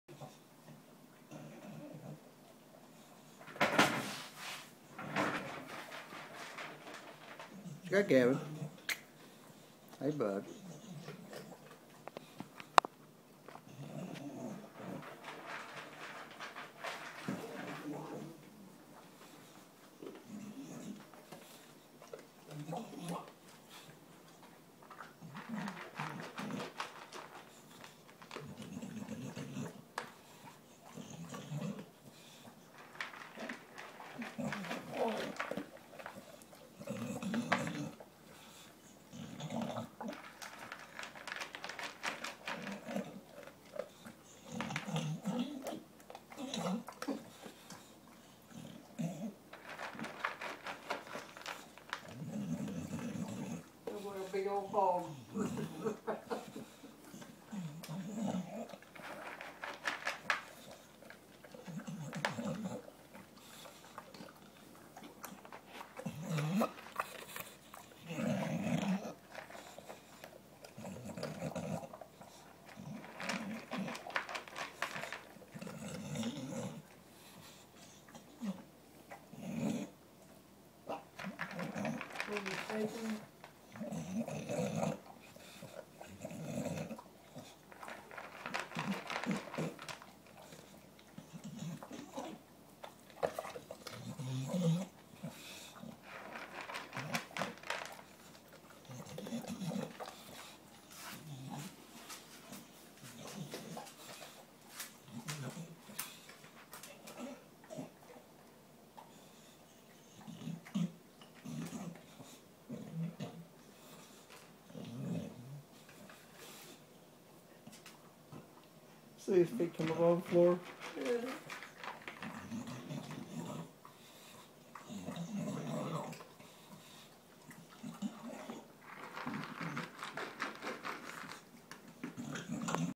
Gavin having dinner
Gavin tries to eat his food before any of the other dogs get to it. And he us such a good and clean eater.